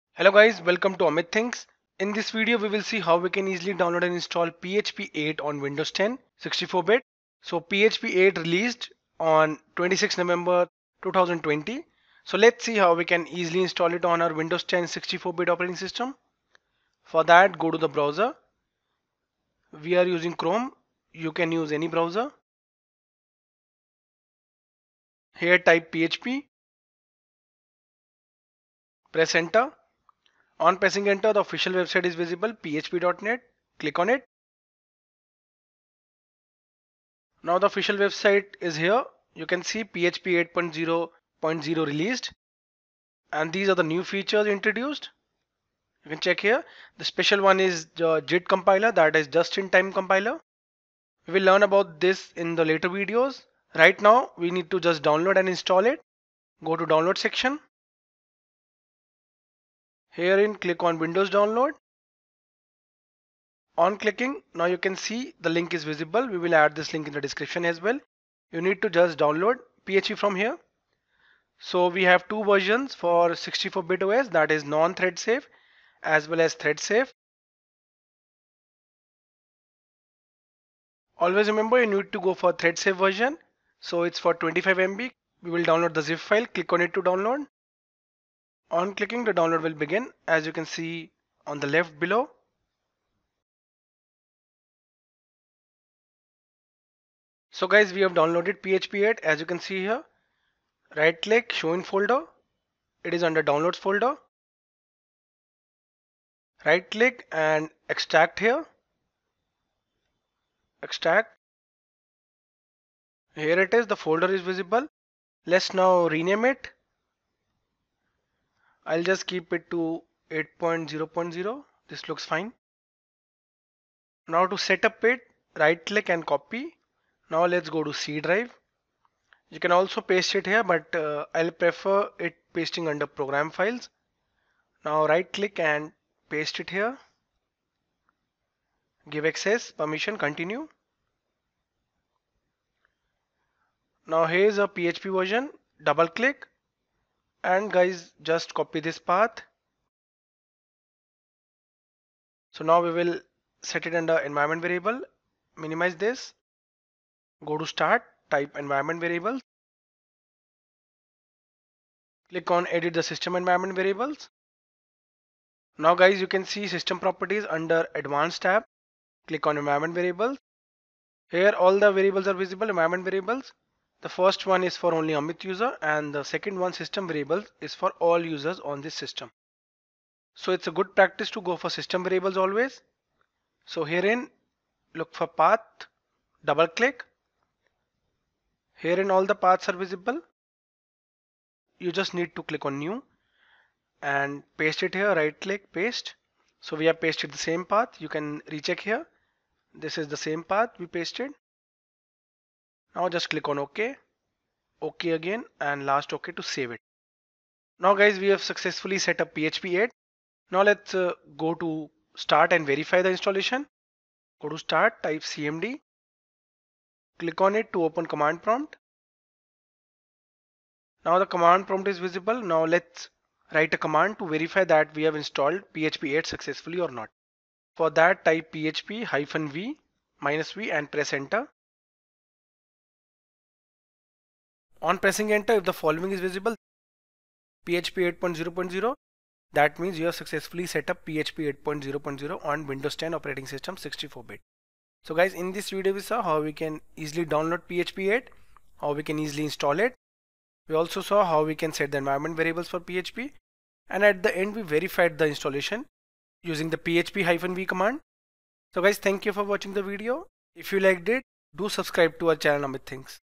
PHP 8 Installation on Windows 10 | 64 bit
Install PHP 8.0.0 on Windows 10 64 bit operating system. With that, we will also see how to set environment variable and verify installation.